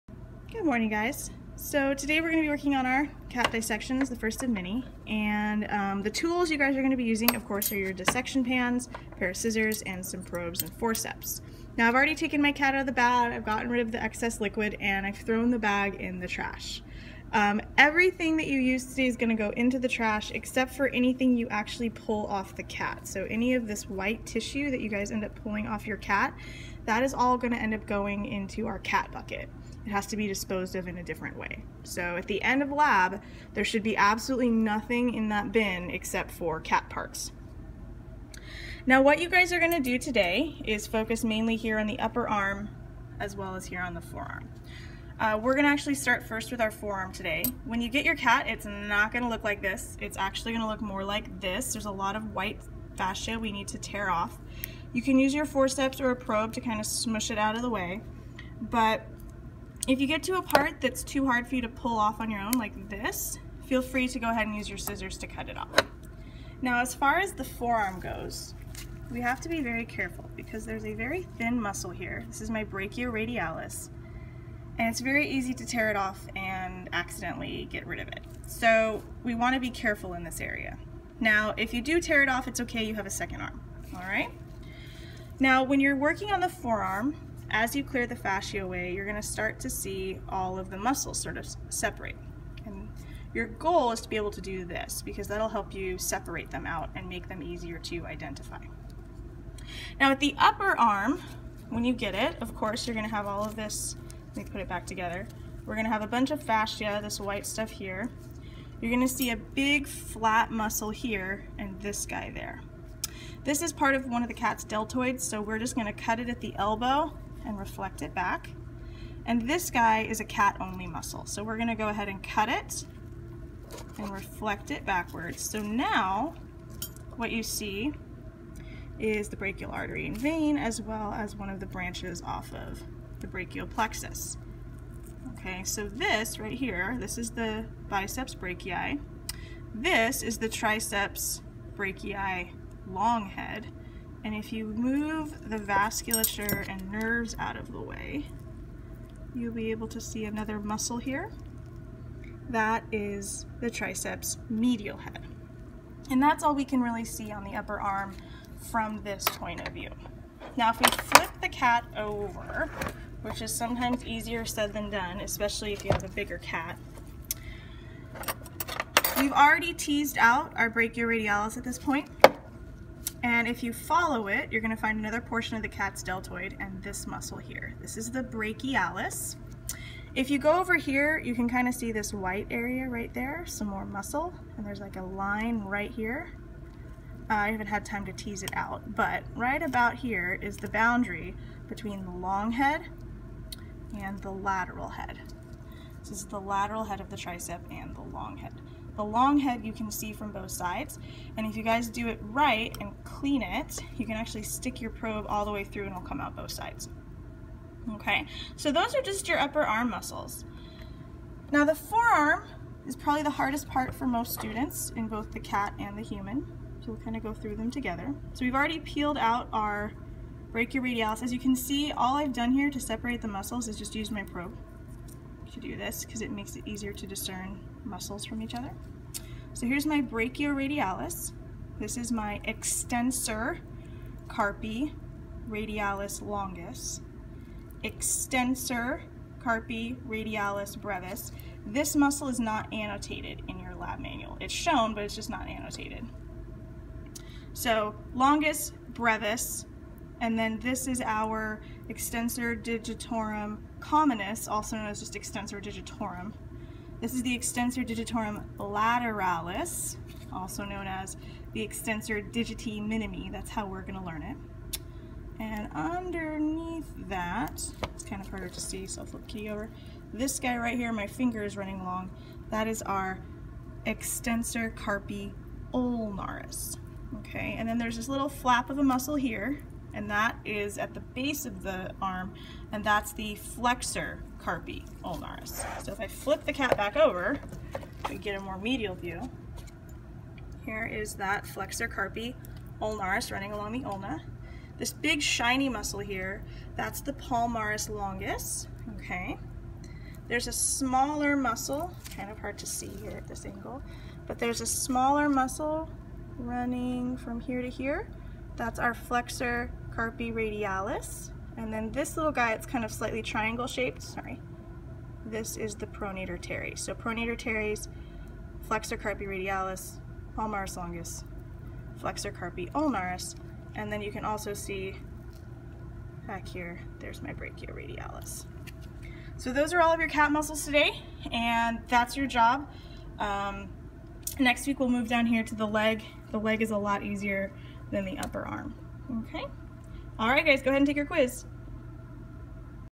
Cat Dissection - Arm
A brief review of the leg muscles we will be dissecting.

Please note the exact muscles we learn have changed since this video was made.